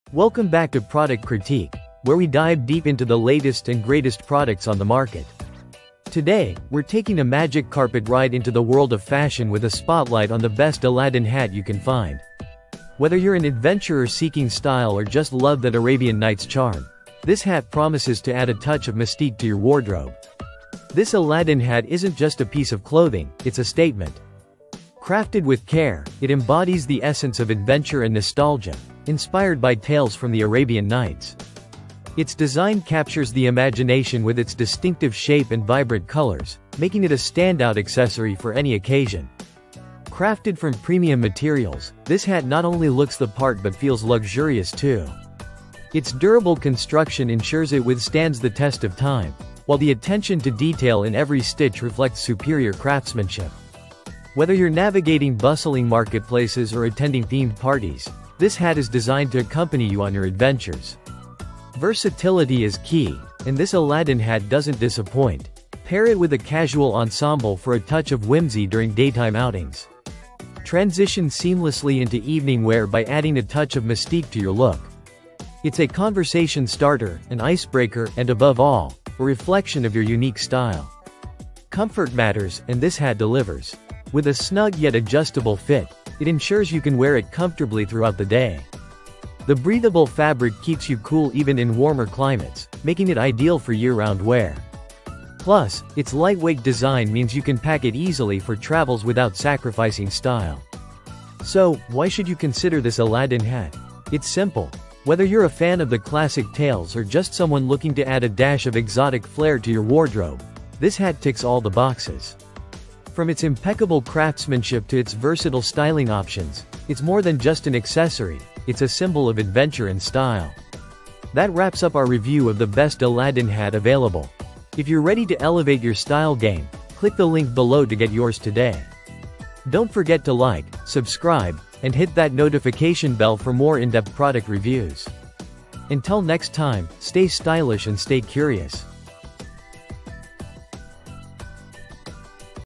🎩 Skeleteen Arabian Red Fez Hat - Moroccan Costume Accessory Fez Hats | Best Aladdin Hat 🎩
Amazon Product Name : Skeleteen Arabian Red Fez Hat - Moroccan Costume Accessory Fez Hats

Transform your look with the Best Aladdin Hat! This iconic accessory captures the enchanting style of the beloved character from the classic tale, making it perfect for costume parties, themed events, or even just adding a touch of magic to your everyday outfit. The Best Aladdin Hat features a comfortable fit and vibrant colors that will make you feel like you're stepping right out of Agrabah. In this video, we’ll showcase the hat’s design, provide styling tips to complete your Aladdin-inspired look, and share ideas for accessories that pair perfectly with it. Join us to discover why the Best Aladdin Hat is a must-have for fans of all ages!